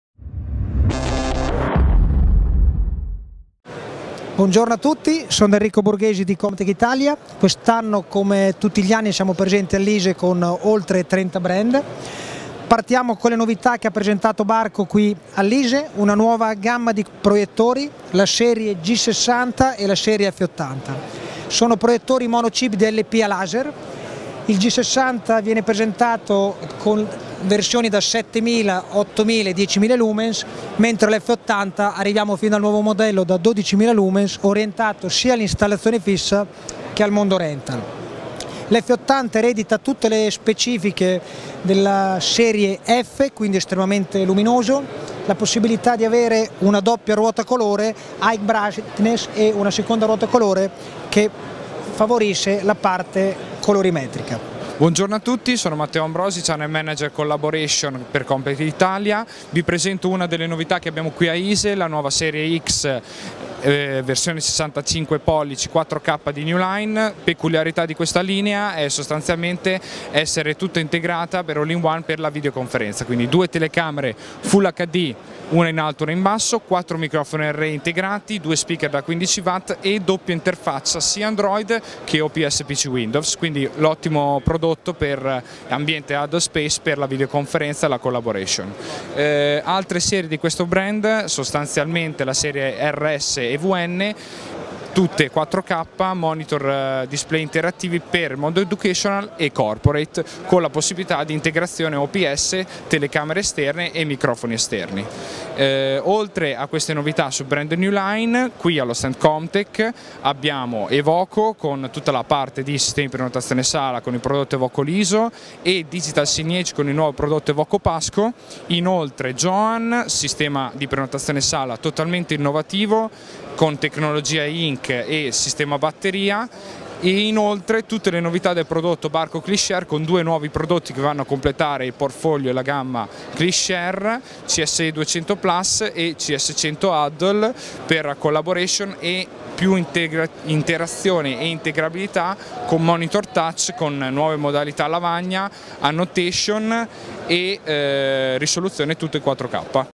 Enrico Borghesi e Matteo Ambrosi, Comm-Tec Italia
L'ISE 2019 di Comm-Tec è ricco di novità, dai principali marchi AV Pro, che abbiamo approfondito insieme ai due manager italiani.